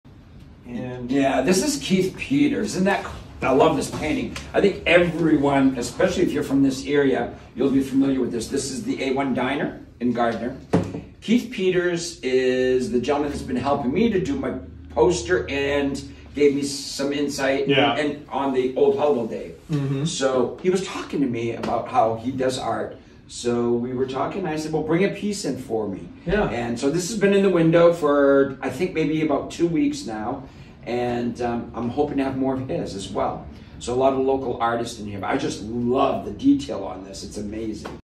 Keith Peters art at Michel-Paul Artist Studio
Keith Peters premiere display of his art at Michel-Paul Artist Studio in Hallowell, Maine.